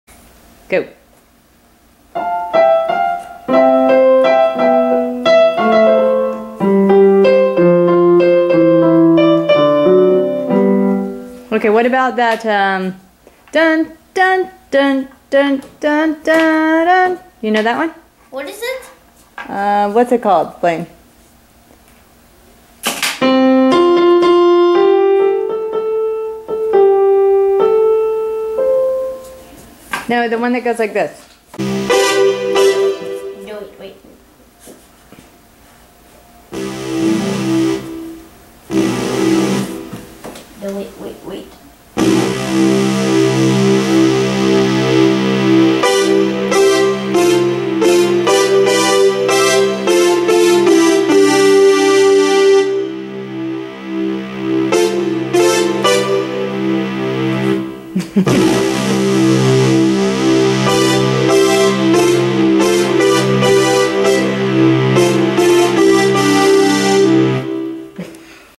Brie playing keyboard
This is my youngest daughter. She has autism and she has never had one piano lesson. This is just a light hearted funny video. She received a keyboard for Christmas and this is her playing it. She actually plays by ear, but sometimes she will write down the words on a piece of paper and write the notes to the words after she has played it by ear. She always amazes me the way her brain works!